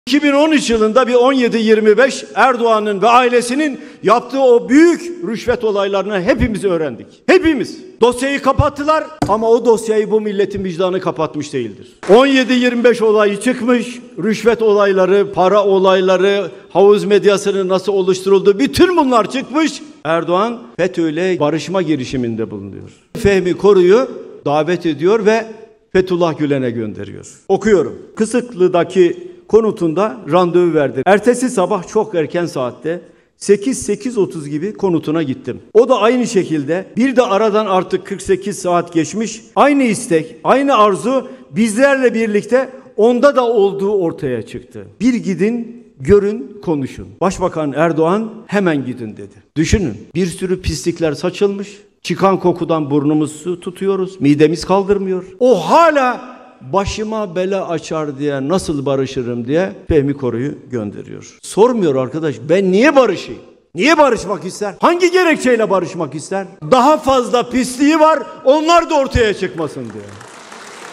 “Bir sürü pislik ortaya saçılmış, çıkan kokudan burnumuzu tutuyoruz, midemiz kaldırmıyor''
Genel Başkanımız Kemal Kılıçdaroğlu:
“Bir sürü pislik ortaya saçılmış, çıkan kokudan burnumuzu tutuyoruz, midemiz kaldırmıyor. O hâlâ barışmak için Fethullah Gülen’e adam gönderiyor. Çünkü...”